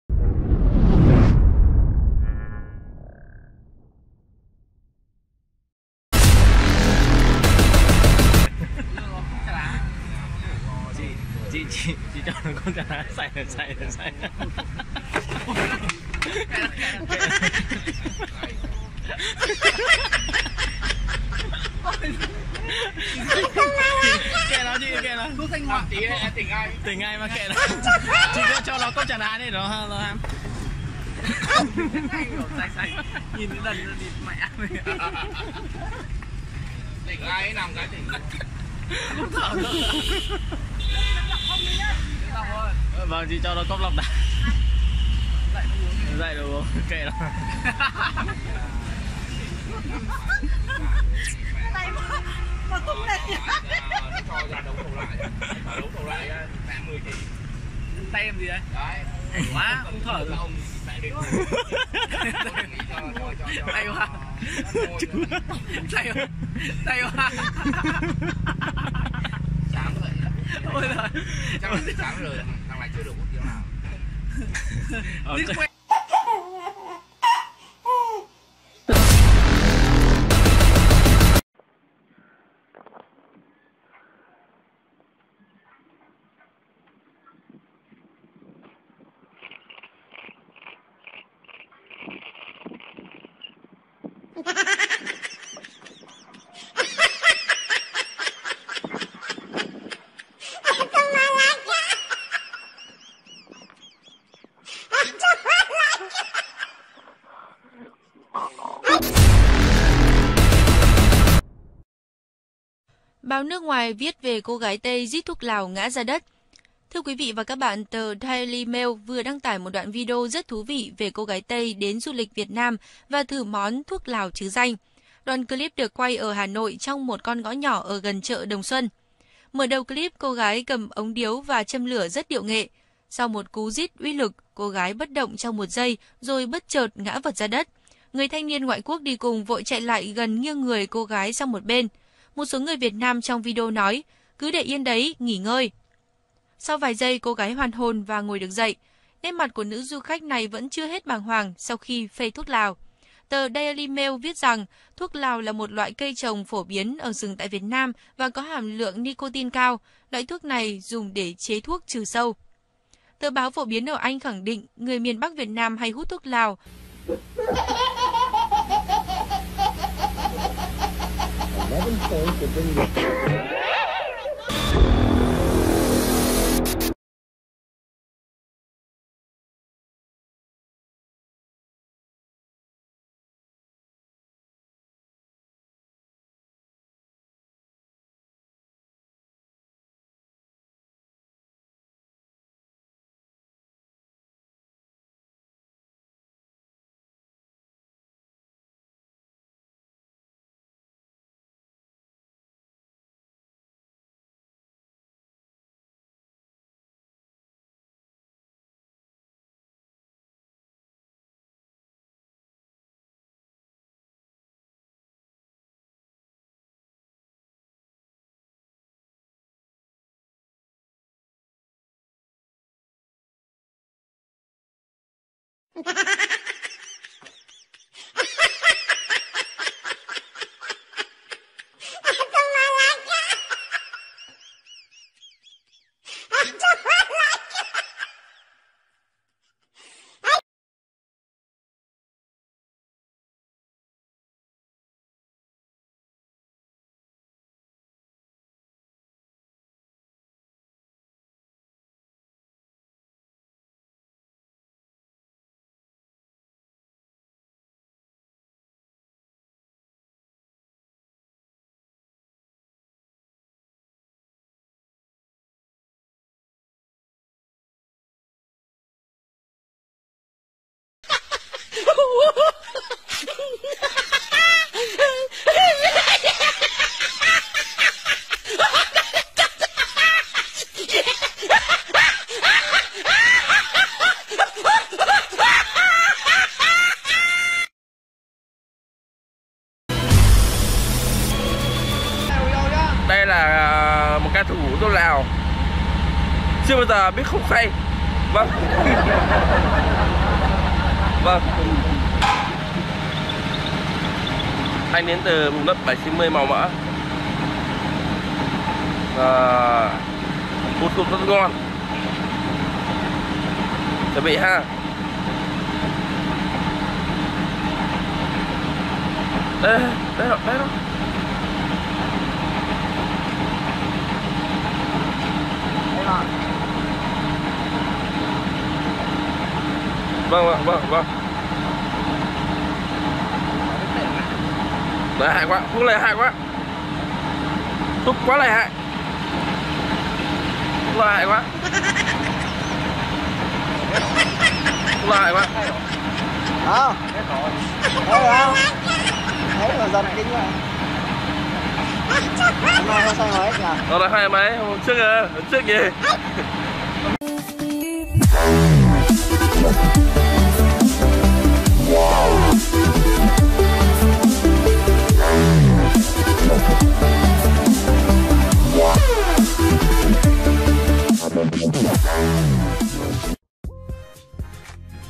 Hút thuốc Lào và cái kết cười đau ruột | Smiling Vietnam and laughing at the heart
- Muôn kiểu hút thuốc và say thuốc lào của người Tây và Việt Nam cười đau bụng với mấy người nước ngoài.
- Mời các bạn thư giãn với các kiểu say thuốc nhé, hi vọng mang đến tiếng cười vui vẻ cho mọi người.